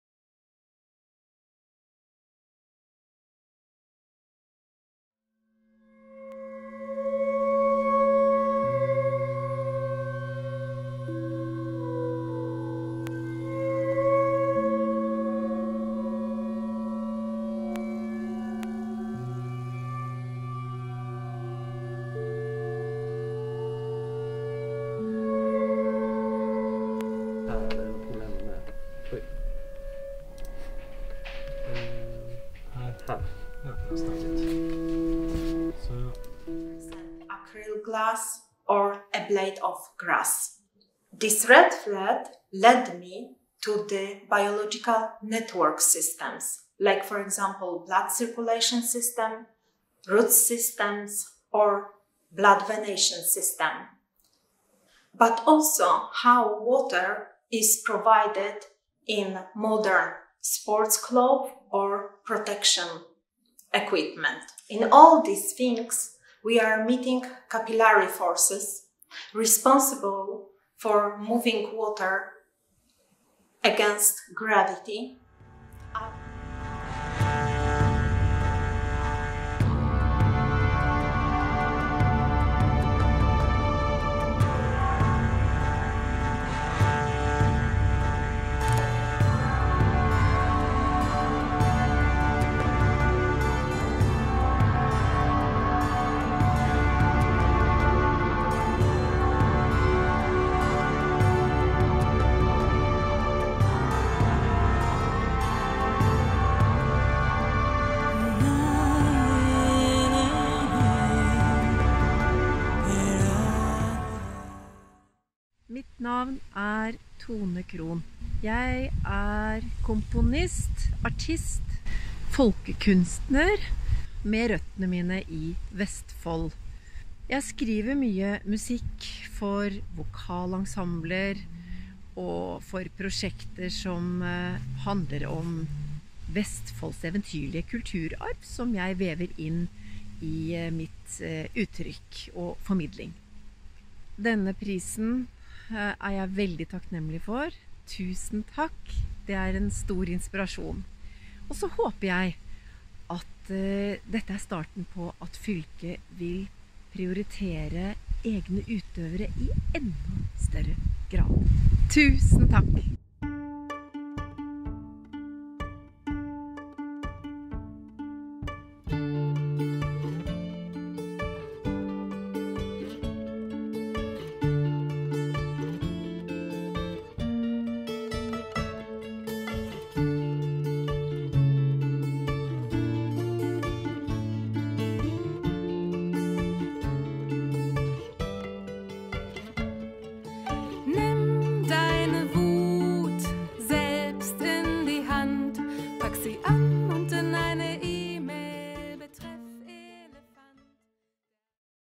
Vestfold og Telemarks kunstnerstipender 2022
Vestfold og Telemark fylkeskommune deler hvert år ut fem kunstnerstipender på kr. 50 000. Stipendene skal bidra til at profesjonelle kunstnere i fylket vårt skal ha mulighet til videreutvikling og kompetanseheving.

Her en presentasjon av årets stipendmottakere:
Tone Krohn, folkekunstner/komponist
Izabela Jadwiga Solcinska, billedkunstner
Nina Ossavy, scenekunstner
Johanna Seim, musiker
Jo David Meyer Lysne, komponist/musiker